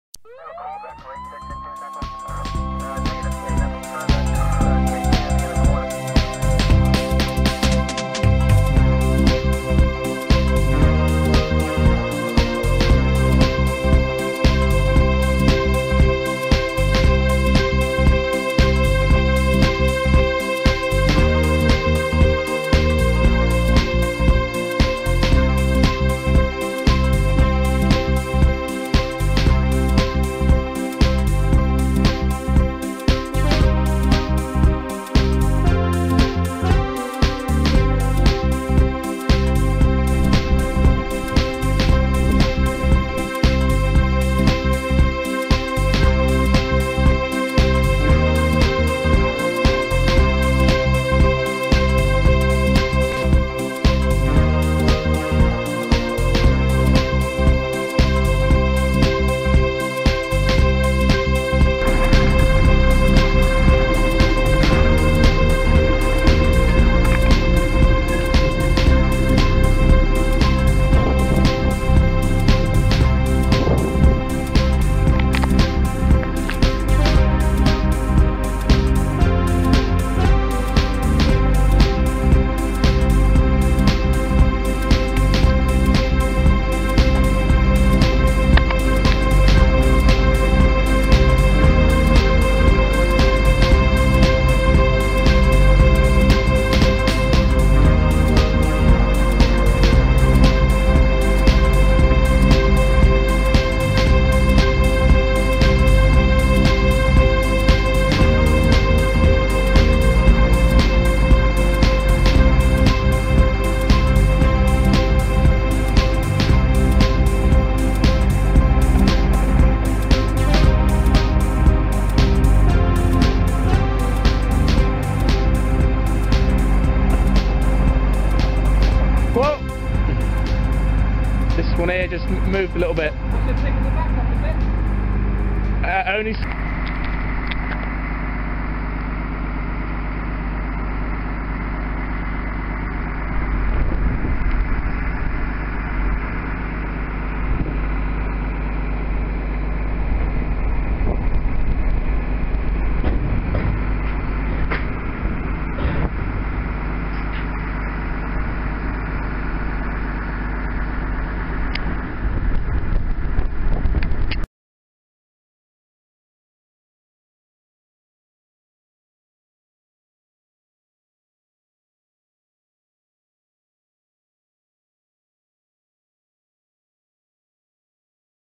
Fishing Boat lifting with Hiab
This is a short filming of craning a Jersey fishing boat. Only a few inches up allowing  the blocks to be moved along the keel to complete a keel repair. More videos to follow on fitting a winch and film of the motor etc.